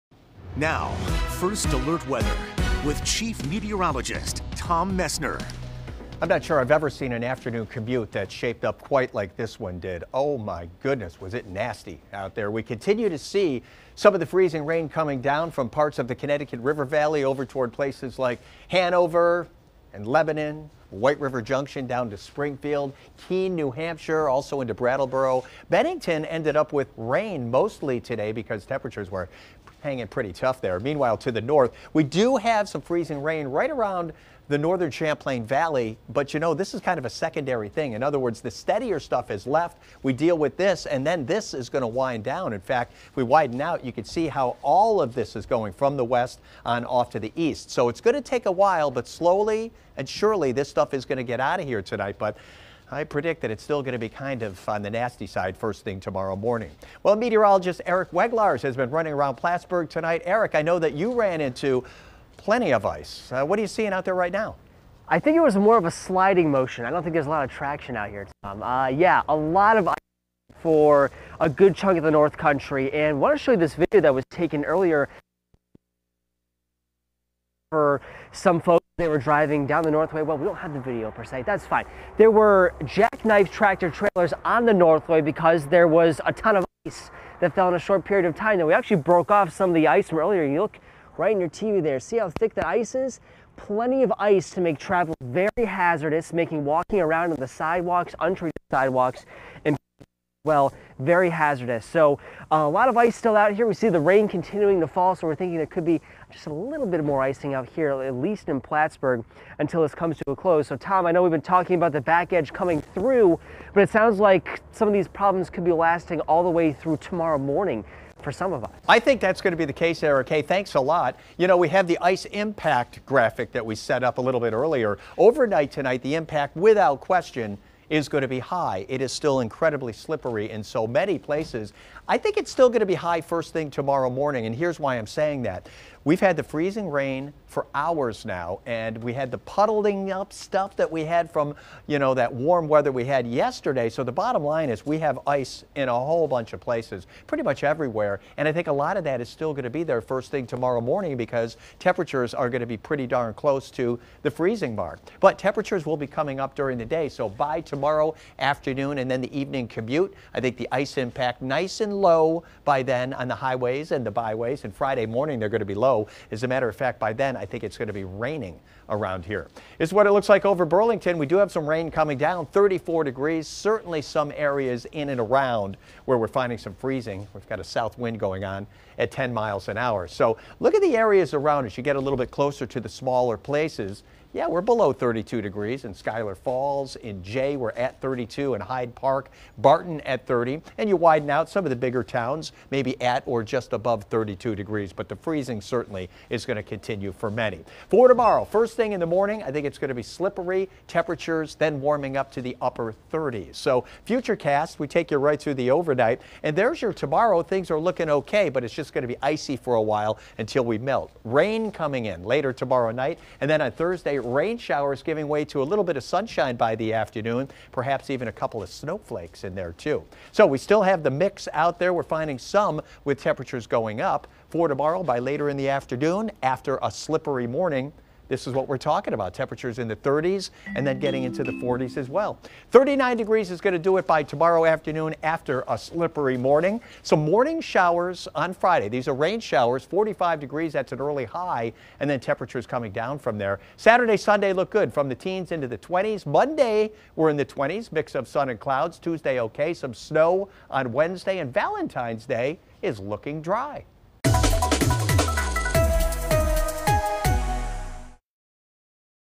Video: Tom Messner is still watching freezing rain. 2.6.19
Temperatures warm Thursday and  begin melting the ice.